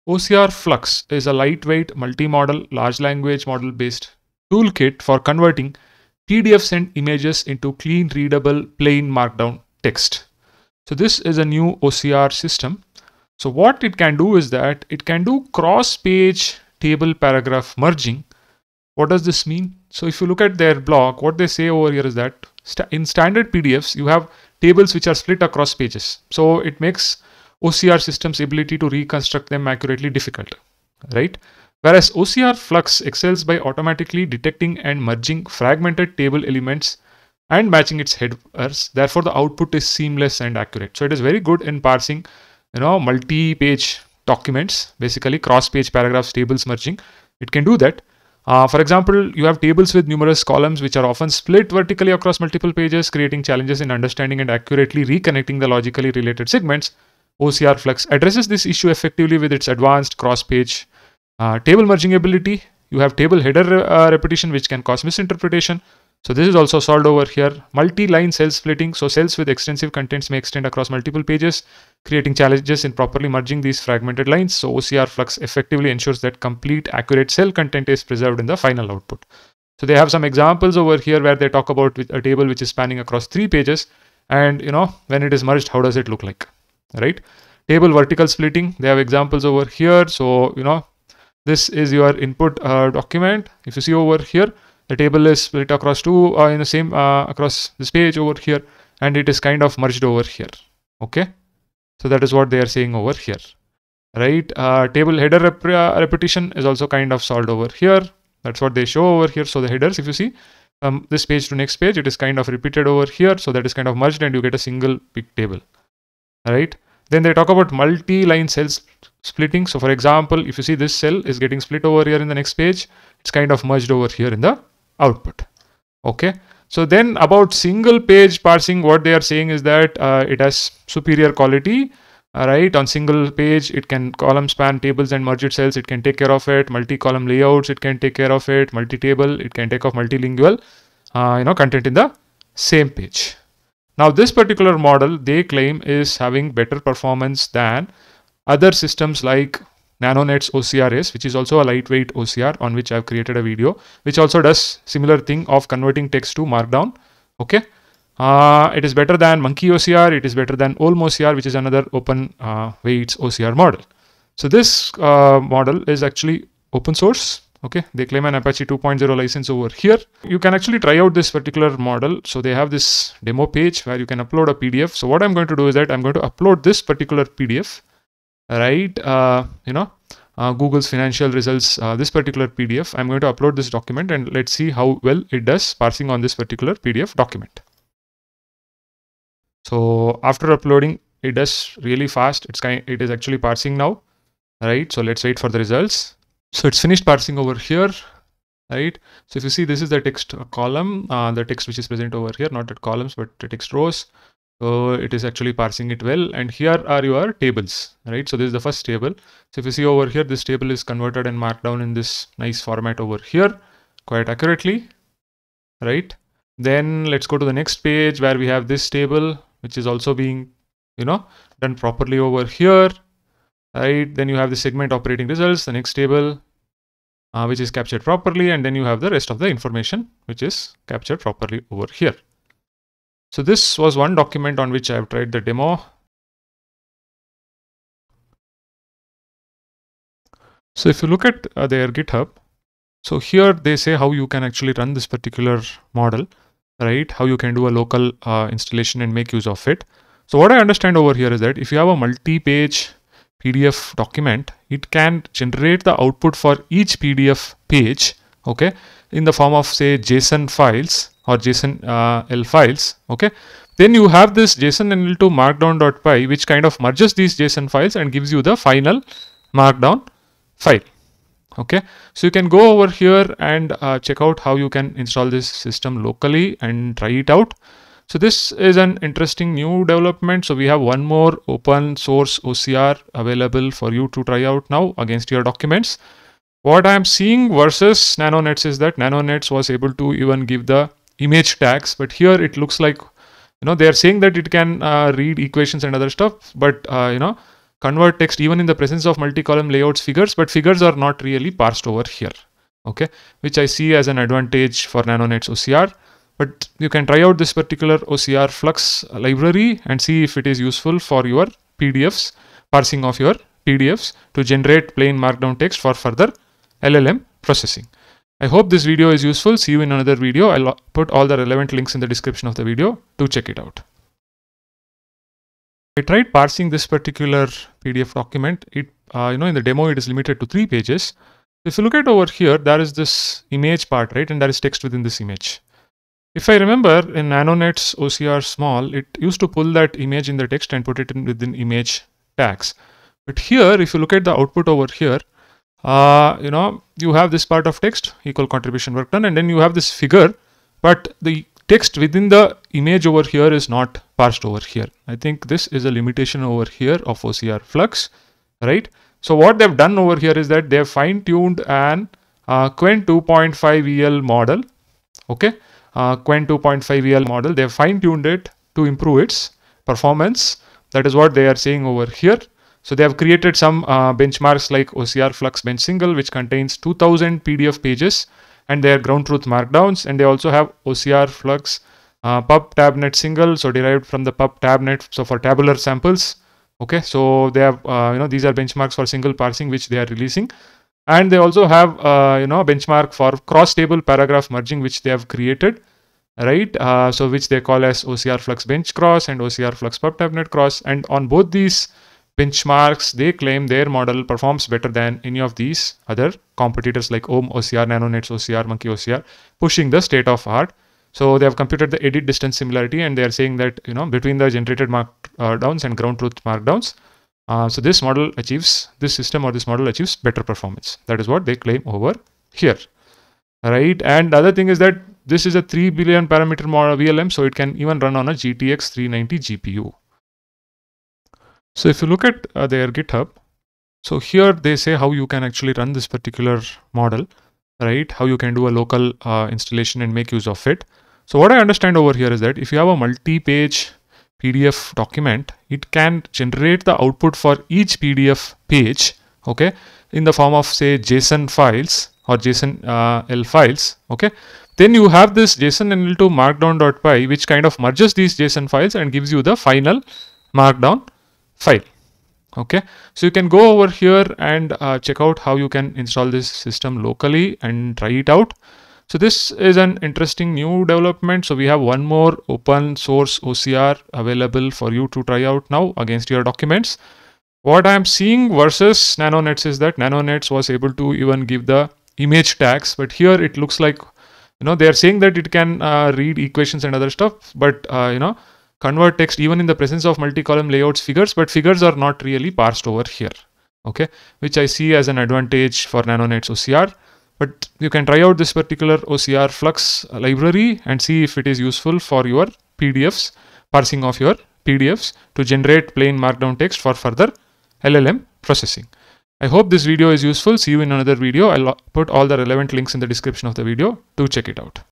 OCRFlux FREE Lightweight PDF/Image to Markdown Text for LLM Processing Toolkit
OCRFlux is a lightweight yet powerful multimodal toolkit that significantly advances PDF-to-Markdown conversion, excelling in complex layout handling, complicated table parsing and cross-page content merging.